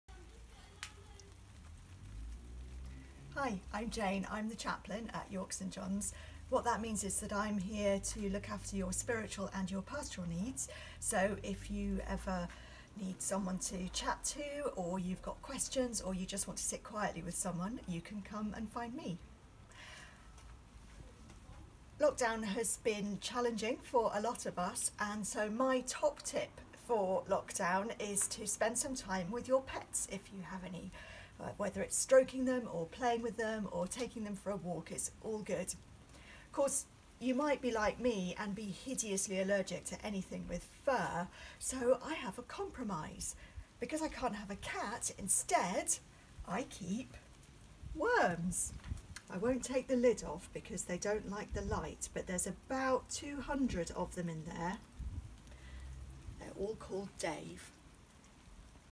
YSJ Top Tips | Meet Jane (Chaplain)
A brief introduction from Jane Speck from the Chaplaincy team.